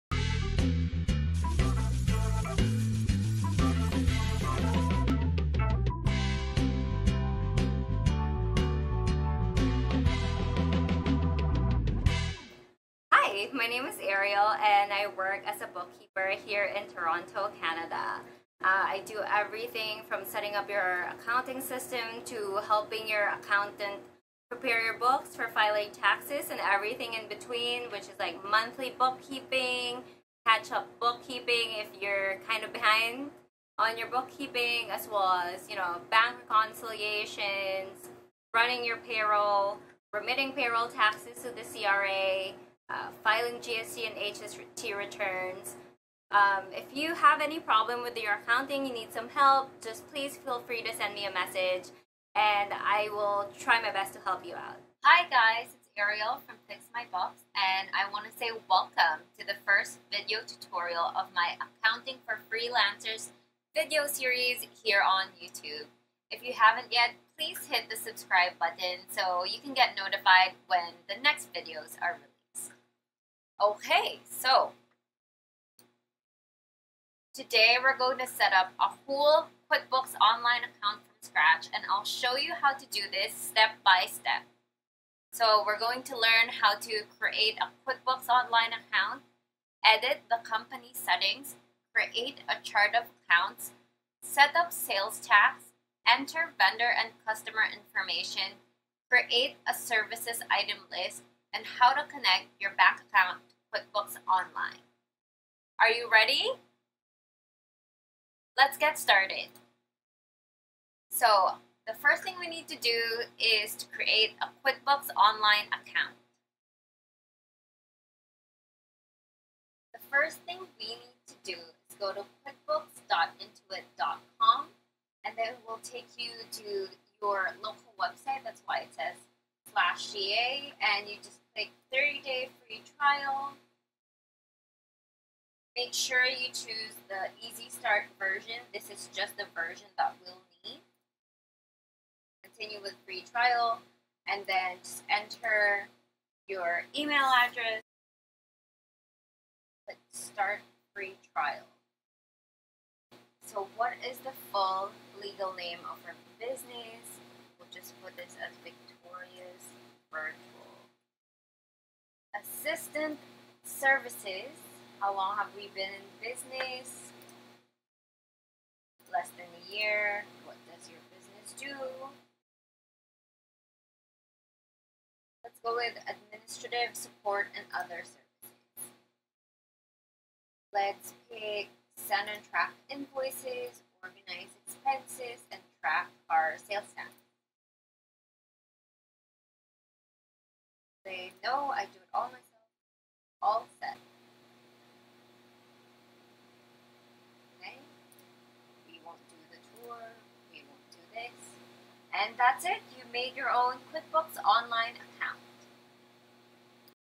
Quickbooks Online Setup - Creating a Quickbooks Online Account
Hi! My name is Arielle and I work as a bookkeeper in Toronto, Canada.

In this video series I will be teaching Freelancers and Small Business Owners how to setup their own Quickbooks Online Account from scratch.